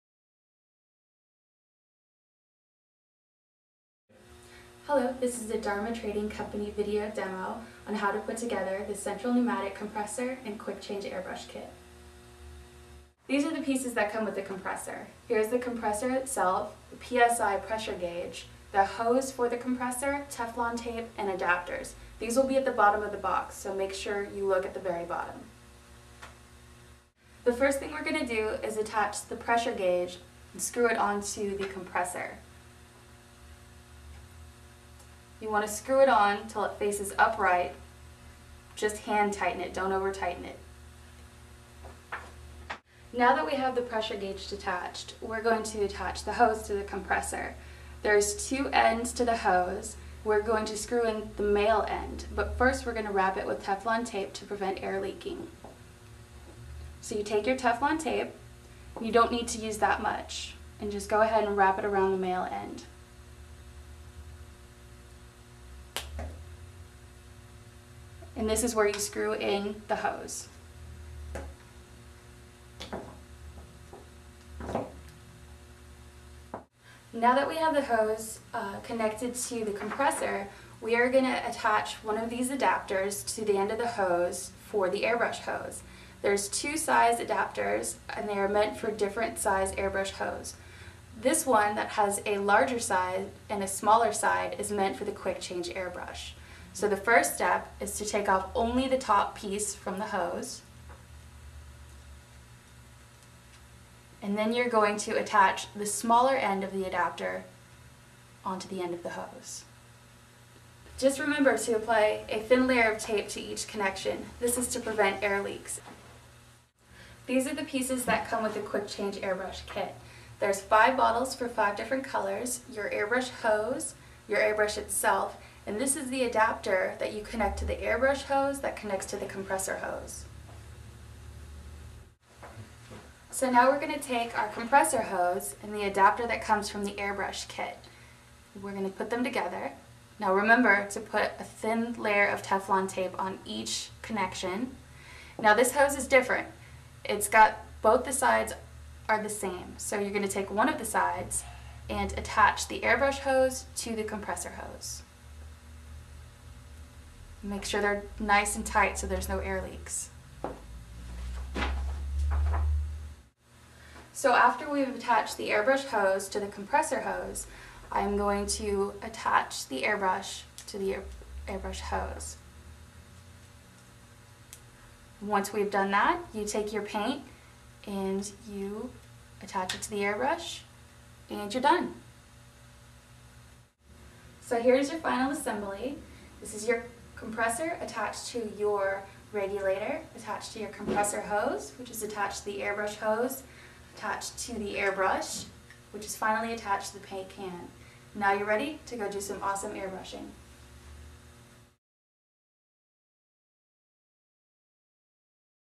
Quick Change Airbrush Kit & Compressor
Dharma Trading Co. product demonstration video for the Quick Change Airbrush Kit and Central Pneumatic 30 PSI Oilless Airbrush Compressor.